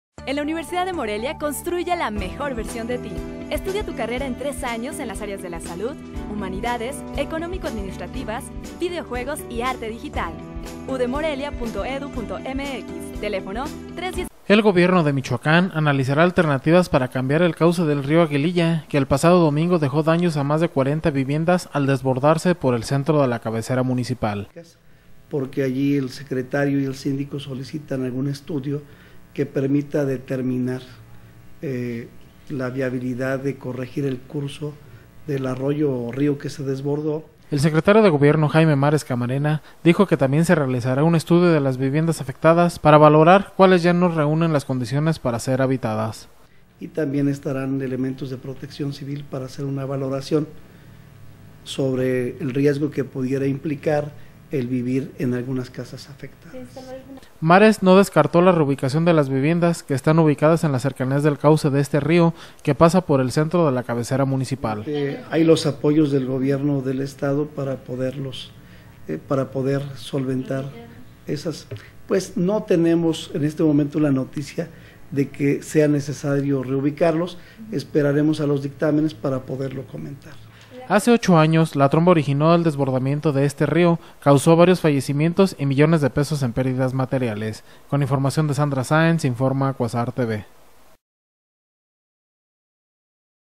Río Aguililla podría ser reencausado, analiza gobierno
El gobierno de Michoacán analizara alternativas para cambiar el cauce del río Aguililla, que el pasado domingo dejó daños a más de 40 viviendas, al desbordarse por el centro de la cabecera municipal...